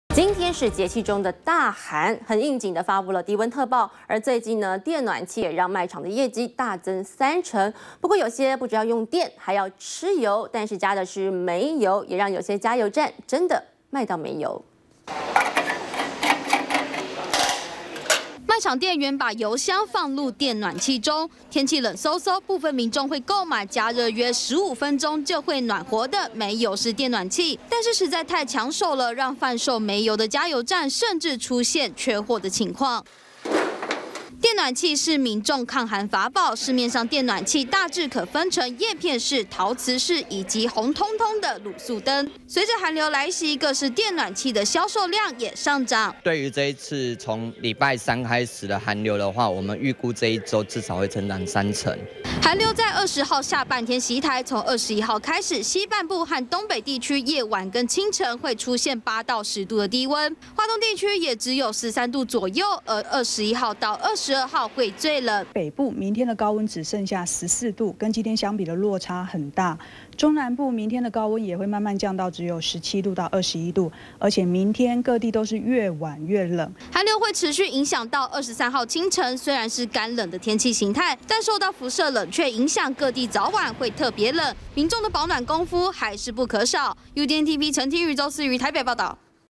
【2014.01.20】冷氣團接連報到 各式電暖器熱賣 -udn tv
寒流又來了，最近電暖器也讓賣場業績大增三成，不過有些不只用電還要吃油，但是加的是煤油，有些加油站還真的賣到沒油。完整內容 , FB , Twitter , G+